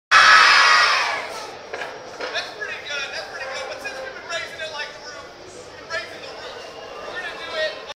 Disneyland Jedi show
A video I took just before the Star Wars Jedi Training show